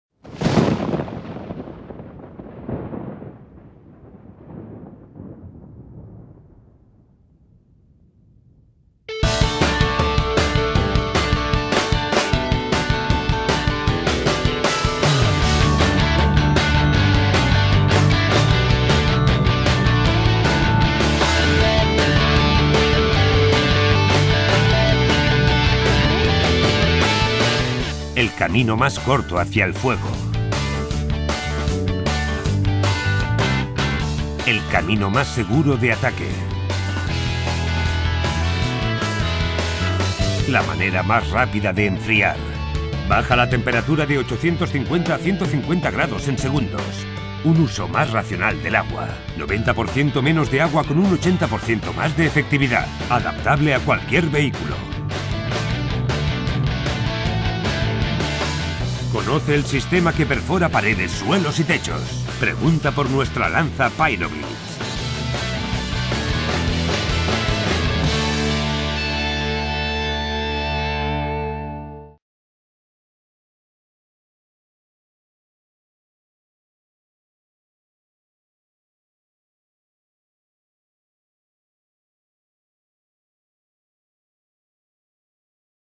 Urbantech - Pyrolance
La innovación de PyroLance
PyroLance® es una solución de nueva generación para acceder, enfriar y extinguir fuego con una efectividad insuperable. Es un producto rompedor que puede utilizarse tanto como herramienta individual como en combinación con la línea de ataque PyroBlitz®. Ambas líneas de mangueras suministran un caudal de agua por encima de 100 BAR o diez veces mayor que la presión de las mangueras tradicionales. También pueden usarse con espumas Clase A y Clase B.